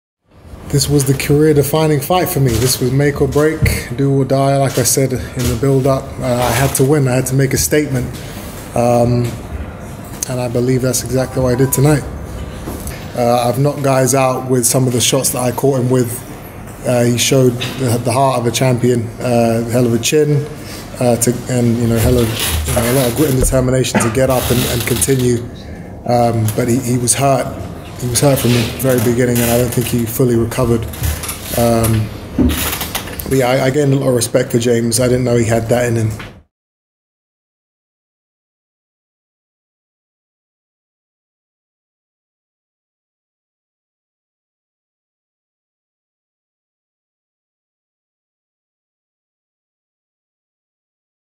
Chris Eubank Jr Says His Win Over James DeGale Was 'Career Defining'
Chris Eubank Jr says his points victory over James DeGale was his "career-defining fight".